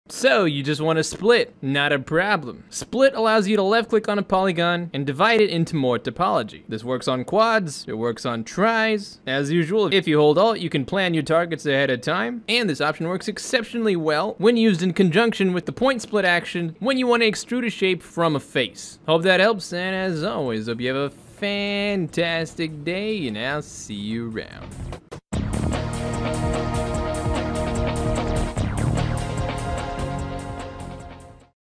ZBrush - ZModeler Polygon(Actions) - "Split"
Split faces is probably one of the lesser used tricks in the ZModeler hand-book, but every now and then it can come in handy - Here's what it does in 30 seconds-!!